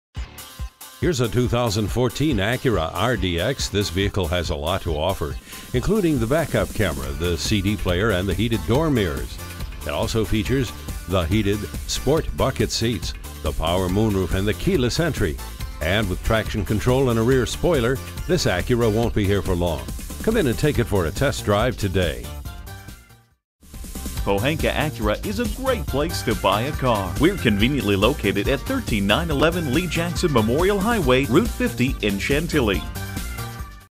2014 Acura RDX Chantilly-Fairfax-VA Washington-DC, MD EL002585 - SOLD
Year: 2014
Make: Acura
Model: RDX
Trim: Base
Engine: 3.5L V6 SOHC 24V i-VTEC
Transmission: 6-Speed Automatic
Color: Brown
Interior: Parchment
Stock #: EL002585

Address:
13911 Lee Jackson Hwy